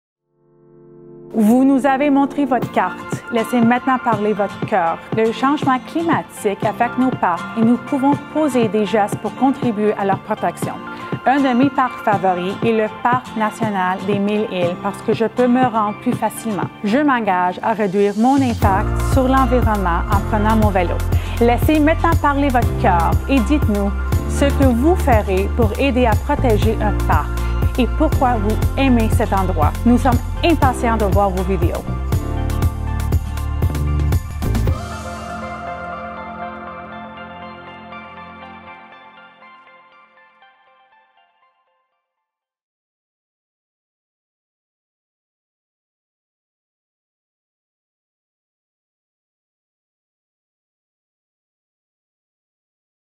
Laissez parler votre cœur
Vous nous avez montré votre carte; laissez maintenant parler votre cœur. Le changement climatique affecte nos parcs et nous pouvons poser des gestes pour contribuer à leur protection. Dites-nous ce que vous ferez pour aider à protéger un parc, et pourquoi vous aimez cet endroit.